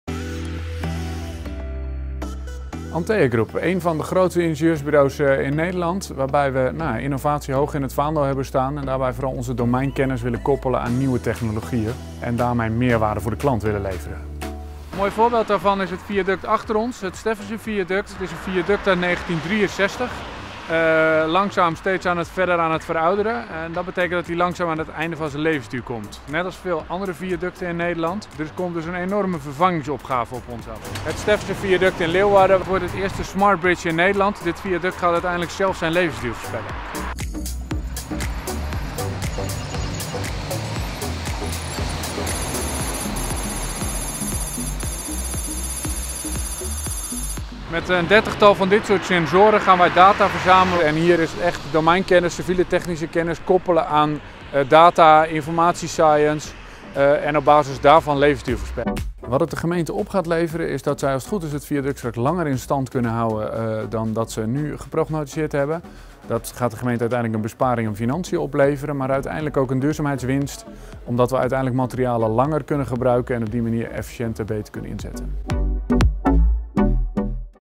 Inzending Antea Group - Categorie: Proces Innovatie
Innovatie: Smart Bridge draagt bij aan betrouwbaar in beeld brengen vervangingsopgave